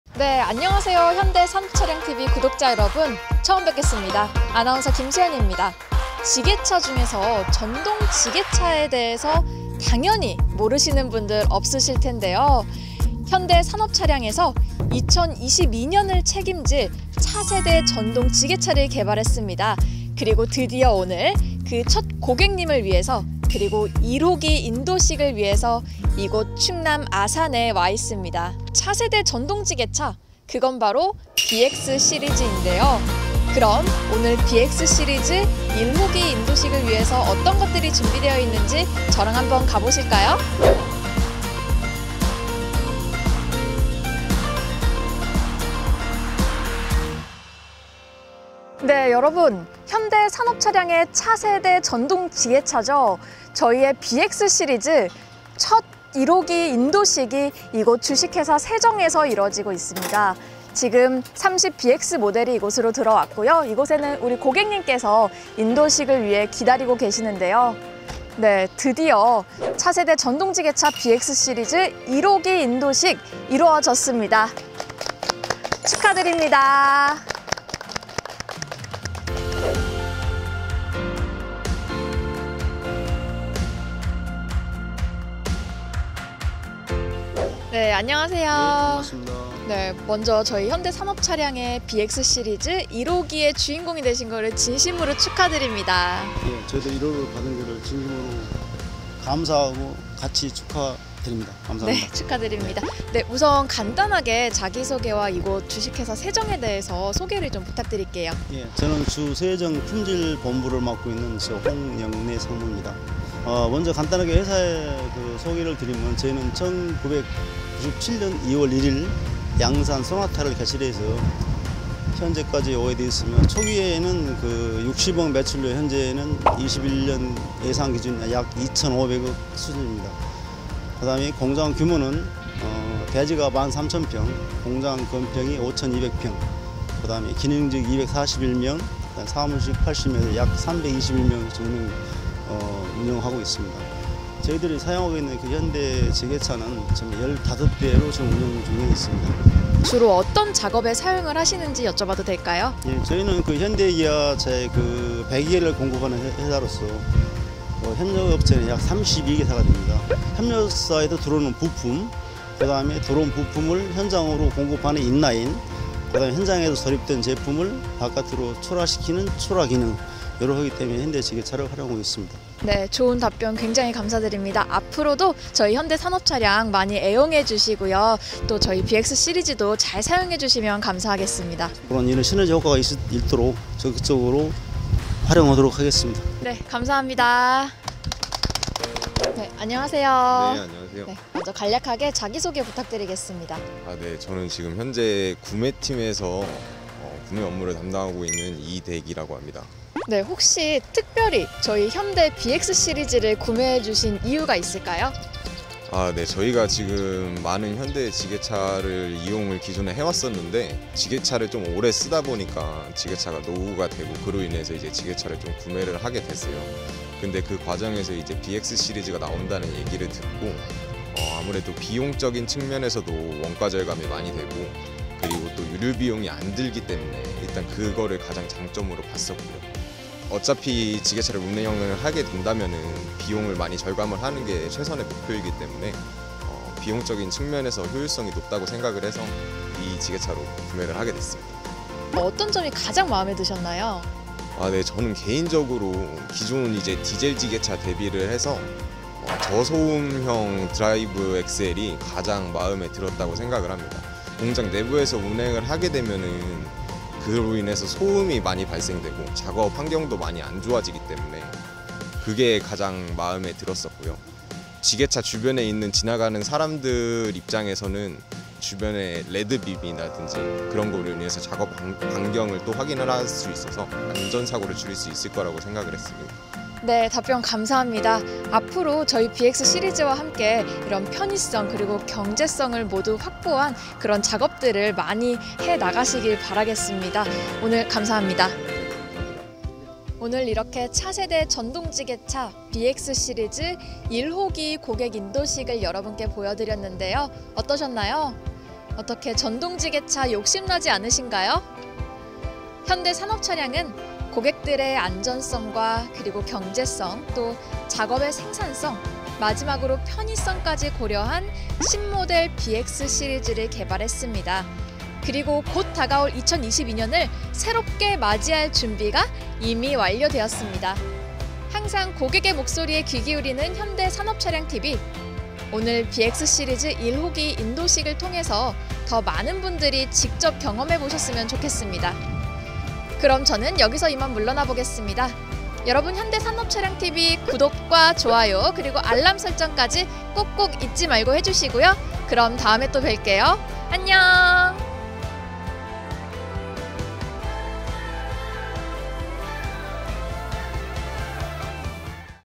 차세대 전동 지게차 B-X 시리즈 1호기 인도식! 첫 주인을 만나다!
안녕하세요. 현대산업차량TV 입니다.

지난 11월 공개된 차세대 전동 지게차 B-X 시리즈 1호기가
드디어 첫 주인을 만났습니다!

영광스러운 인도식 현장!
지금 바로 영상을 통해 확인해보세요!